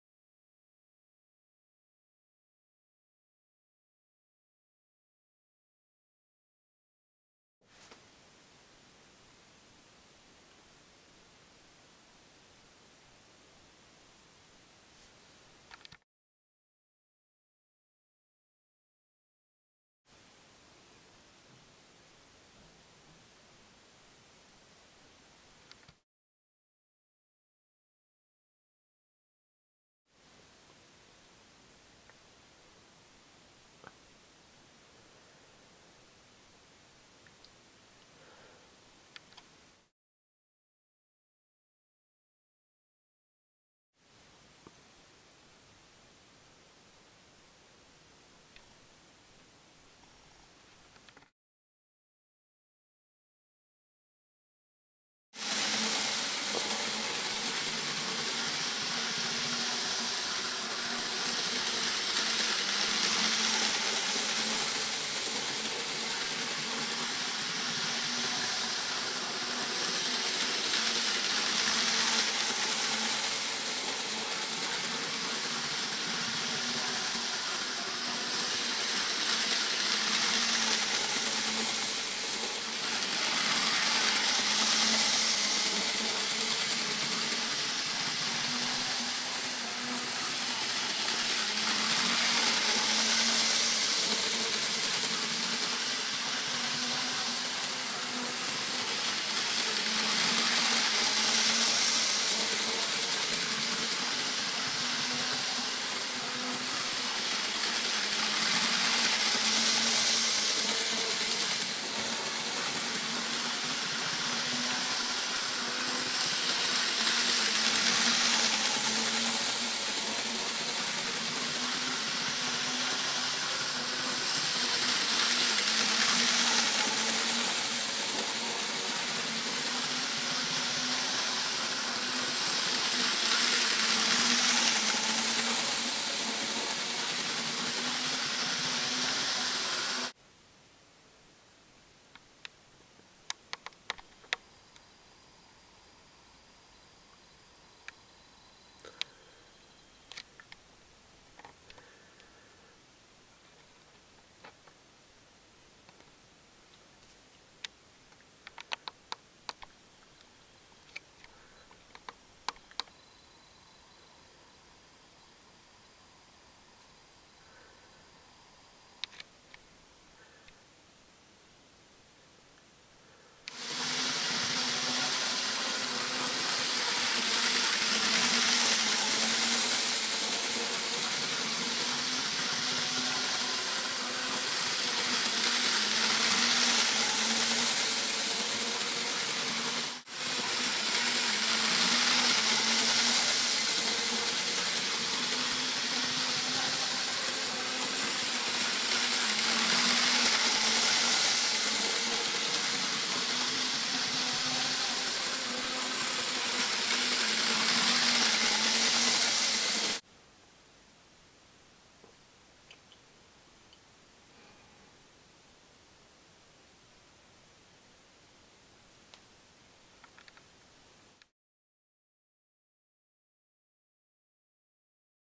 Badgers Green Part 2
The 2nd part in a series of building a late 1970's BR blue era N gauge layout. Badgers Green see's the opening of the partly finished Darkwood Tunnel along with the arrival of class 52 Western Invader.
Clump Foliage and Earth Undercoat by Woodland Scenics. Plaster Bandages by Noch and Terra-Forma. Sycamore Trees by Scene Scapes. BR Van by Oxford, Blue Circle Lorry also by Oxford. Cows by Noch. Class 52 Western Invader by Dapol. All other locomotives and rolling stock by Graham Farish.